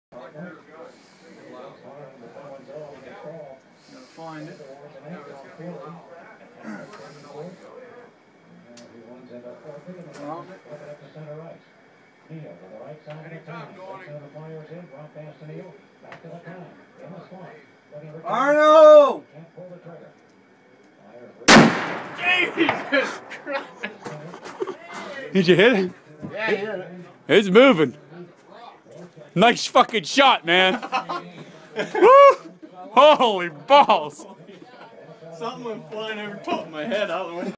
50cal BMG. Brookville PA
Shooting at 300 yd Hardox 400 steel 10" gong. The concussion was huge. In the backgound you can hear the Penguins announcer say "pull the trigger" right before he shot... the universes align :)